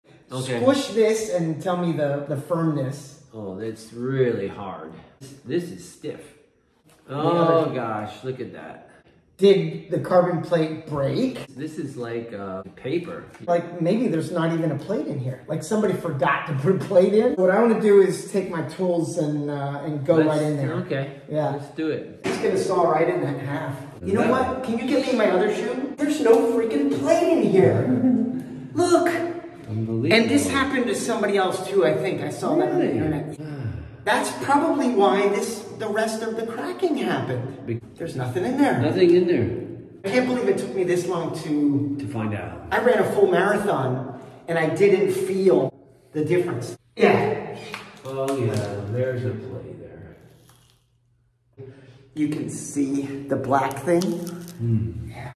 No Carbon Plate in Nike Vaporfly Next%2!
After my sole splitting in my Vaporfly, I tried to Super Glue them, only to find that one shoe had less stiffness.  I cut them open with buddy JC and discovered the lack of a carbon plate!

I don’t think my Vaporfly pair are fakes.  I think someone at the factory just forgot to add the plate into the mold!